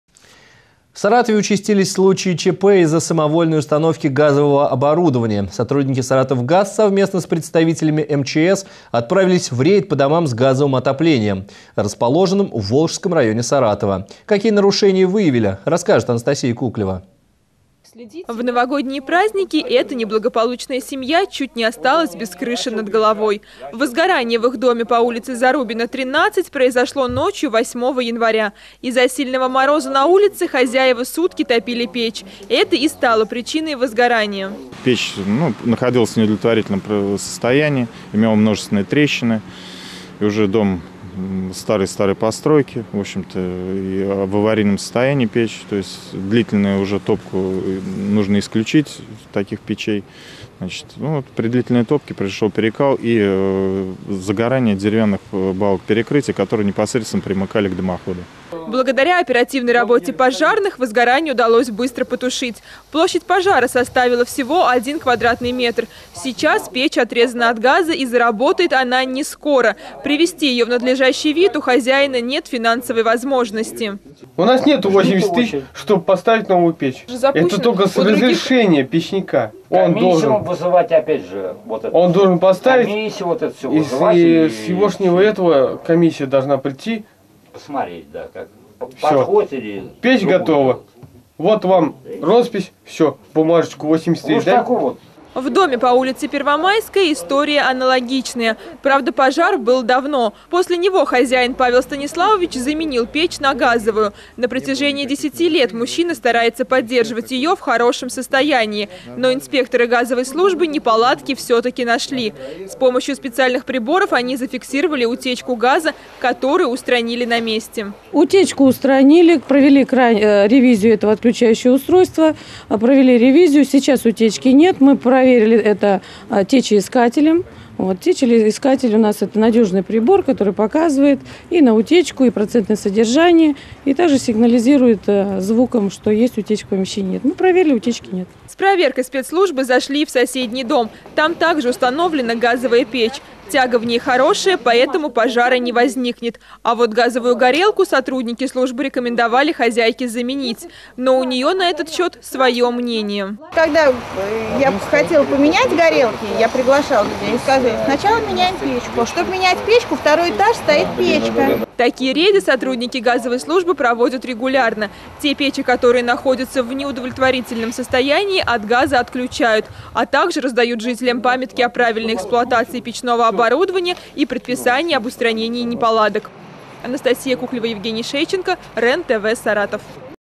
Сотрудники "Саратовгаз" провели рейд по домам с печным отоплением
В Саратове участились случаи ЧП из-за самовольной установки газового оборудования. Сотрудники "Саратовгаз" совместно с представителями МЧС отправились в рейд по домам с газовым отоплением, расположенным в Волжском районе Саратова. Какие нарушения выявили расскажет Анастасия Куклева. Сюжет вышел в эфир 14 января.